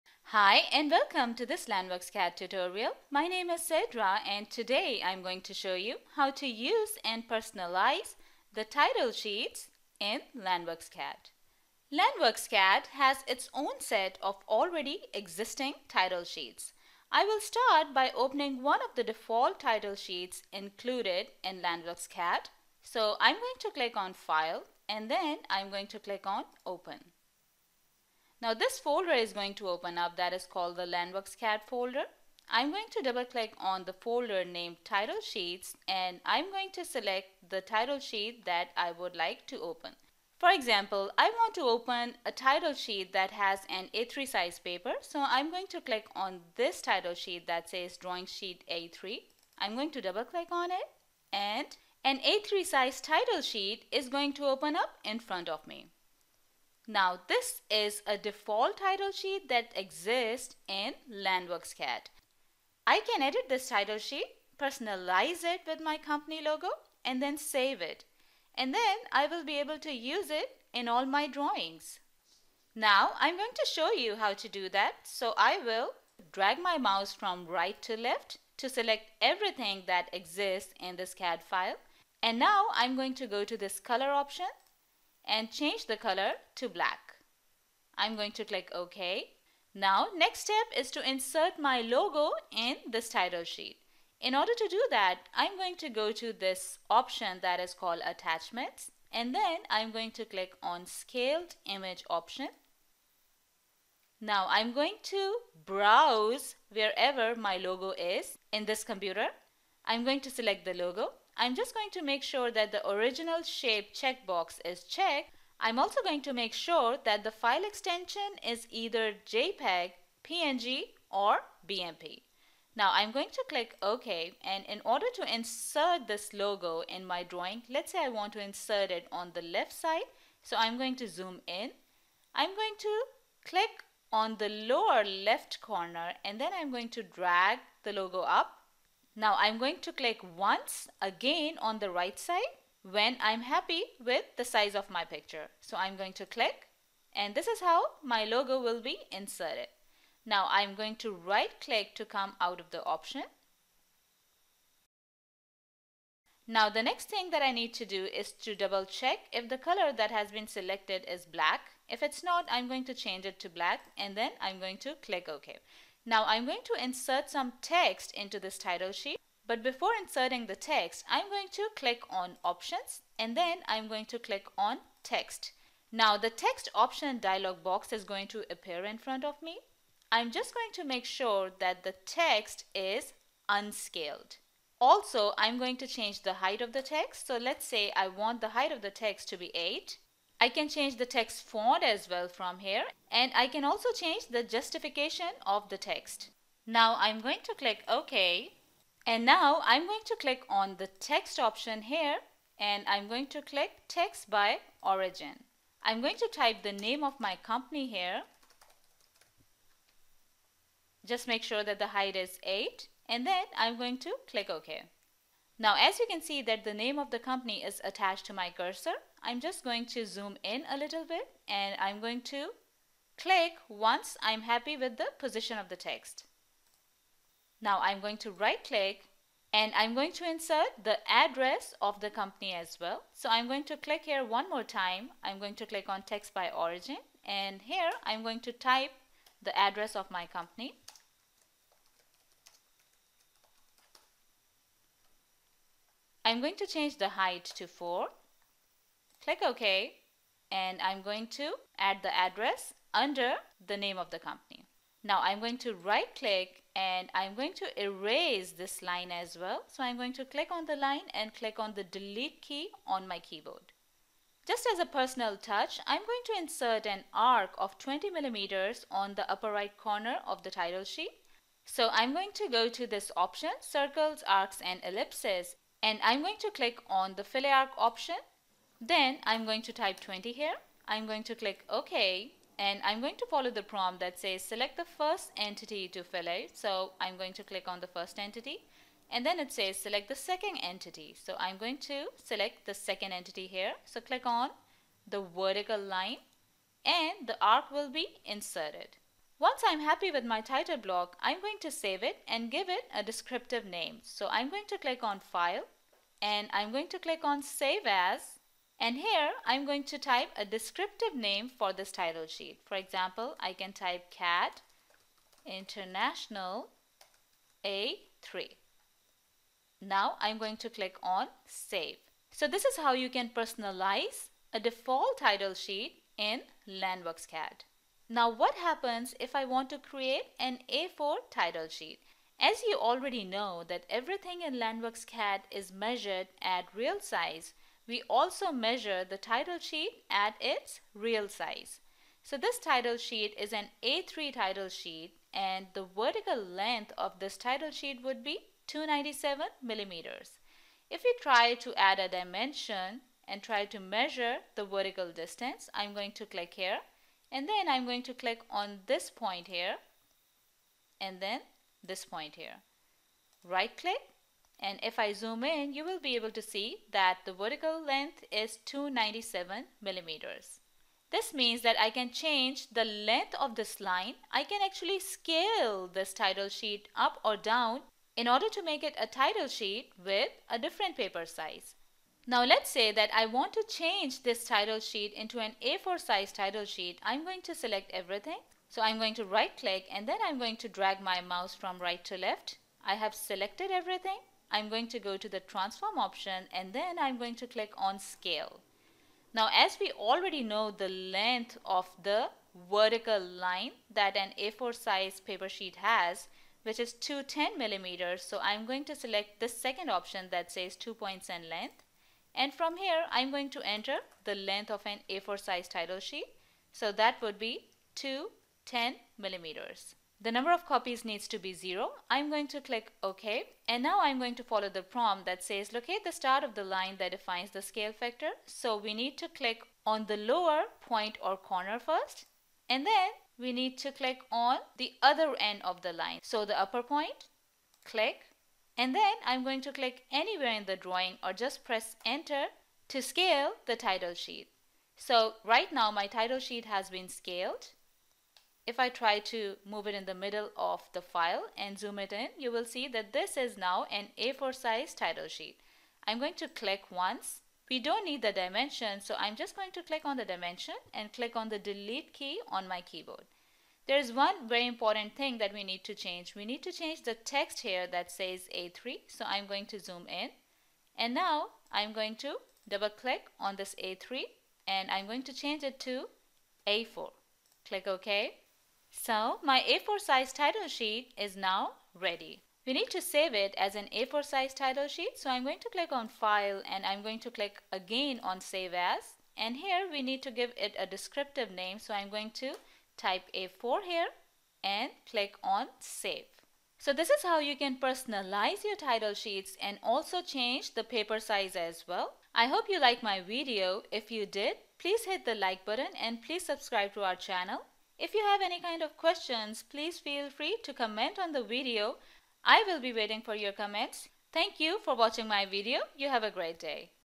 How to work with Title Sheets in LANDWorksCAD-v7
This video with show you how to create your own Title Sheets and Personalize them in LANDWorksCAD- v7